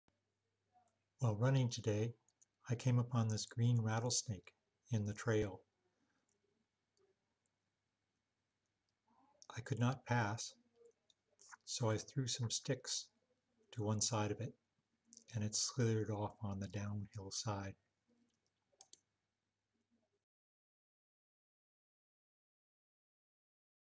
Green Rattlesnake seen today on Zinfandel Trail
I was running this morning on the Zinfandel Trail, about 1.2 miles South of Picchetti Winery when I came upon this green rattlesnake sun-bathing on the trail. I considered turning around, but it was a long ways back.  I chose to throw some small sticks on the uphill side of him/her.  He/she slithered off on the downhill side.